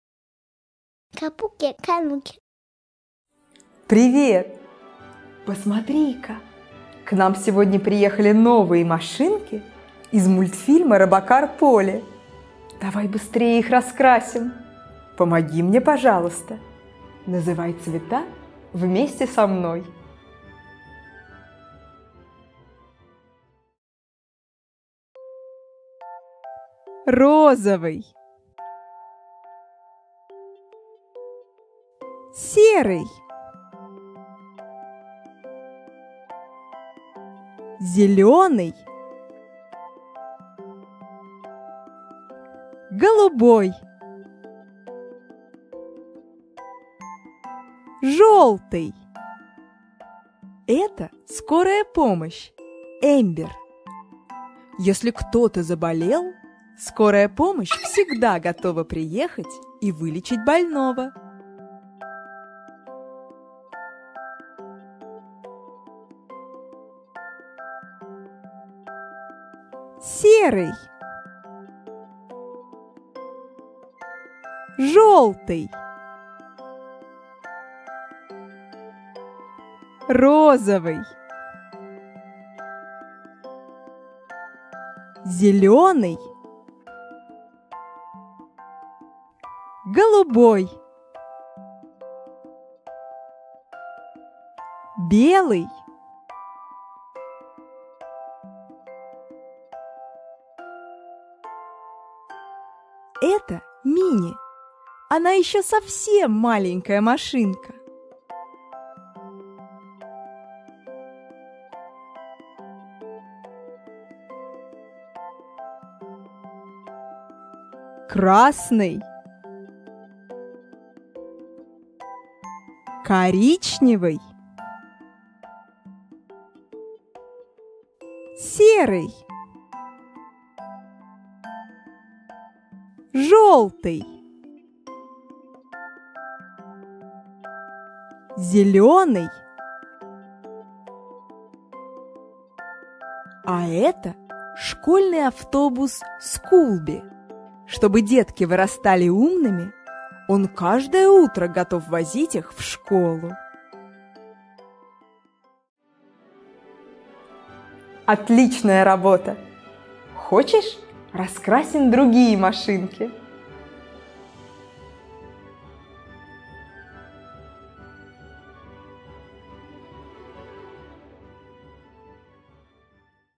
Раскраска из мультфильма - Робокар Поли: скорая помощь, автобус
🌟 Развивающий мультфильм для детей - раскраска из мультфильма Робокар Поли: скорая помощь Эмбер, машинка Мини, автобус Скулби. Давайте вместе раскрасим наших любимых героев и повторим названия цветов!
Смотрите наши мультфильмы на ваших планшетах. Детское приложение для iPAD КапукиКануки
🎈Самые новые серии в приложении Капуки Кануки - скачай и смотри первым